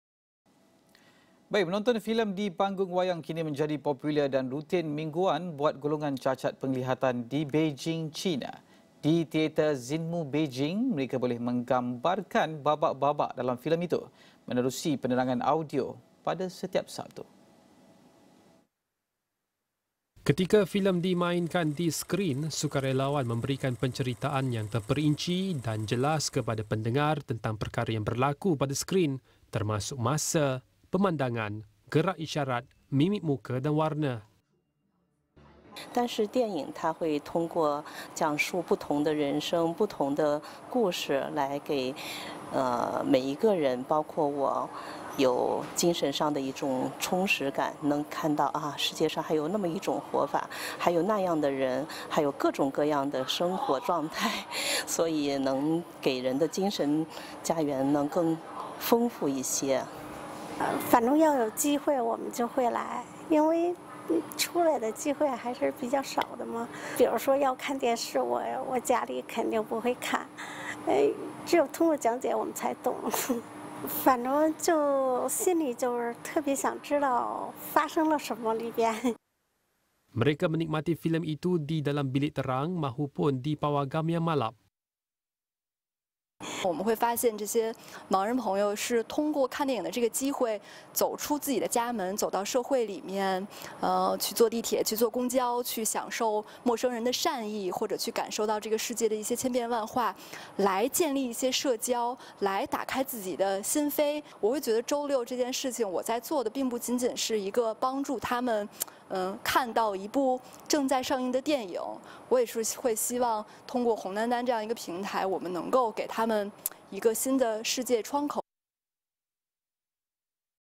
Orang buta di Beijing boleh nikmati pengalaman 'tonton' filem di pawagam
Teater Xinmu di Beijing, golongan cacat penglihatan kini mampu merasai pengalaman menonton filem di pawagam.